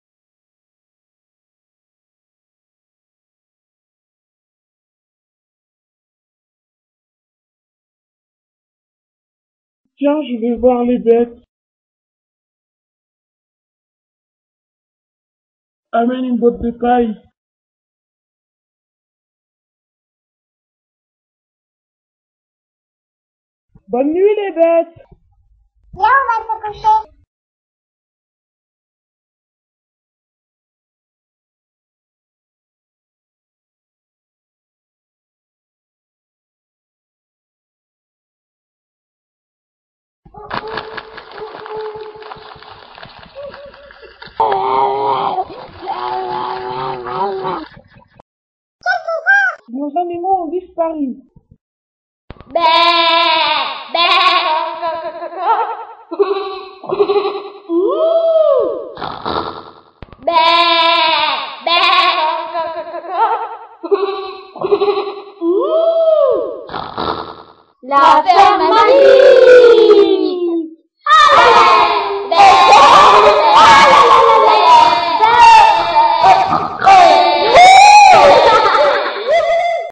La ferme magique.flv
Film réalisé par les élèves d'Anthony Constantin à l'école Jacques Prévert d'Ambazac.
Prise de vue avec MonkeyJam,
Montage avec Windows Movie Maker